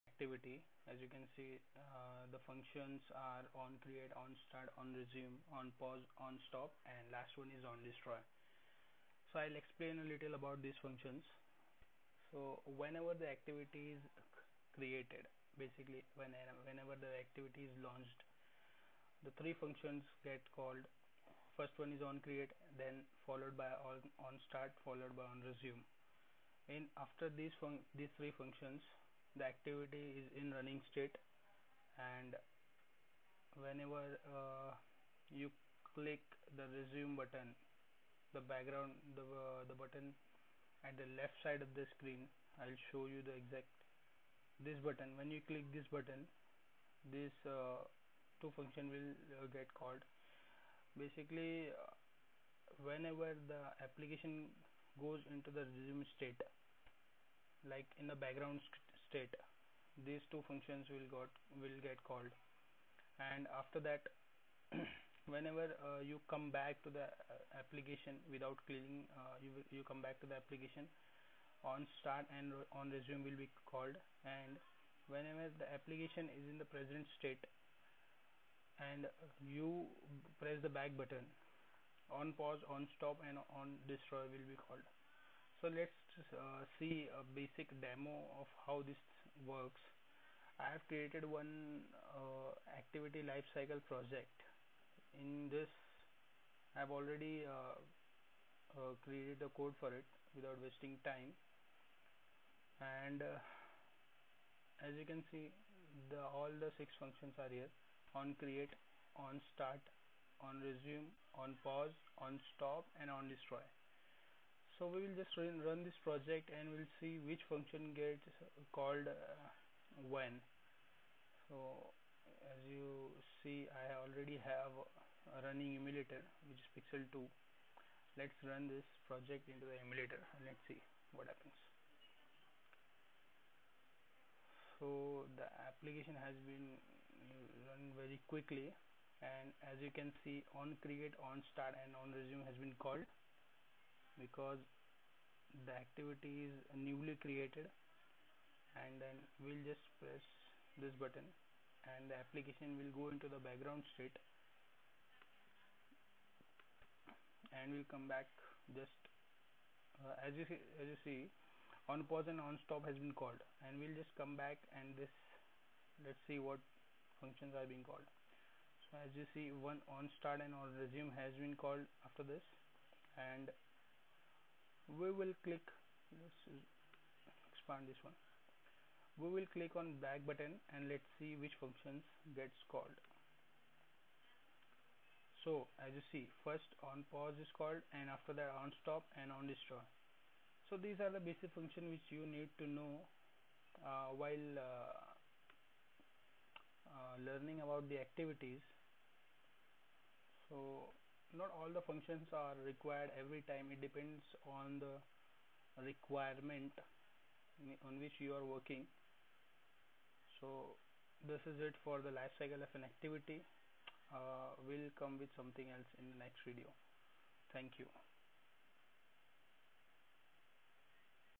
Activity Life Cycle in android
1.Basic life cycle of android activity.
2.Basic explanation of android activity life cycle functions.
3.Android Project demonstration of android activity life cycle.